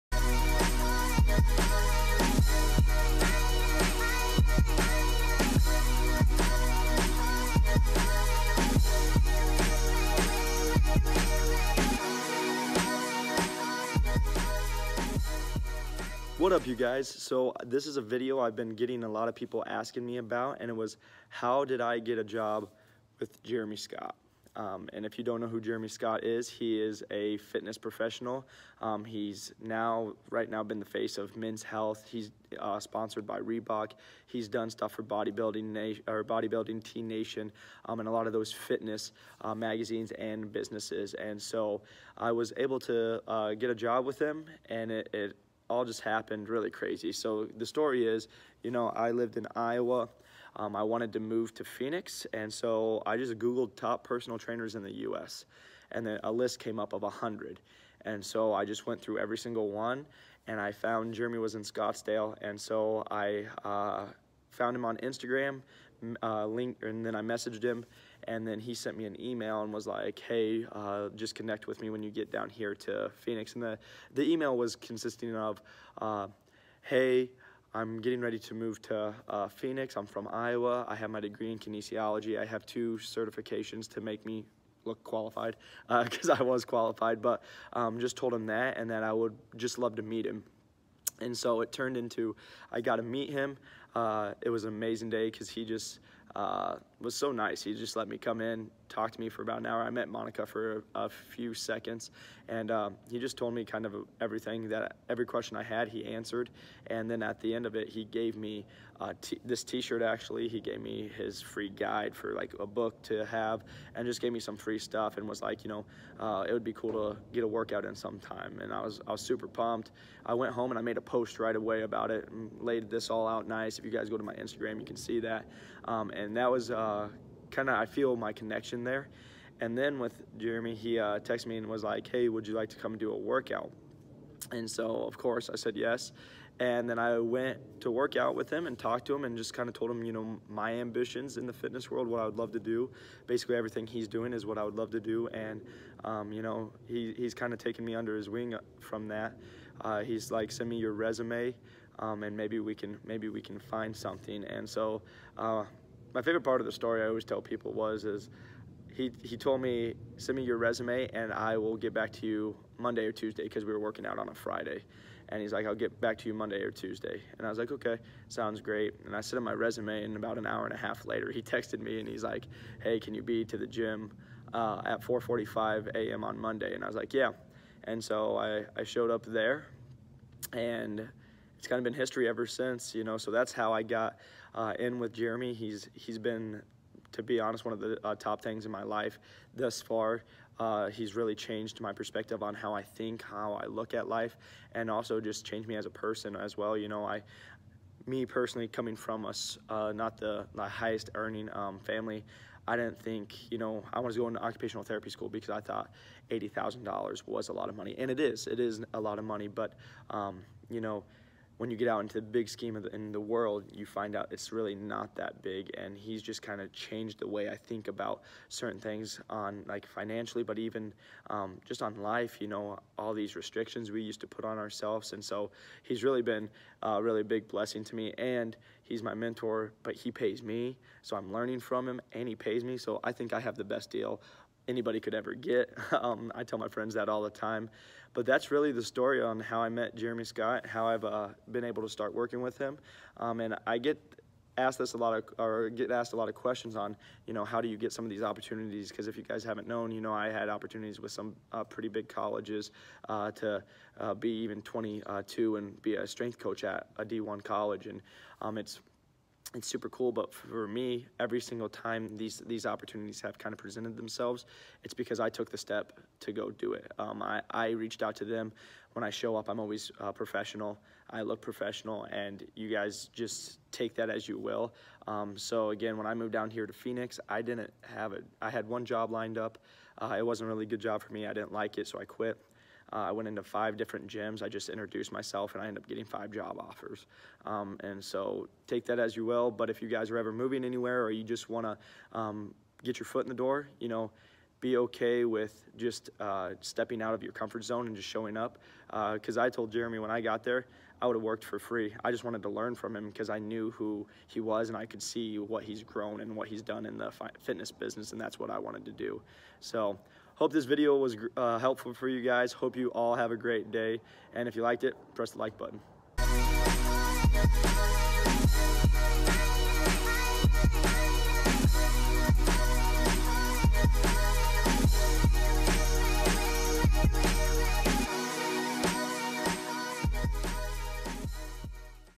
How I Landed a Job with Jeremy Scott
I go over how I landed a job with one of the top Trainers in the Fitness Industry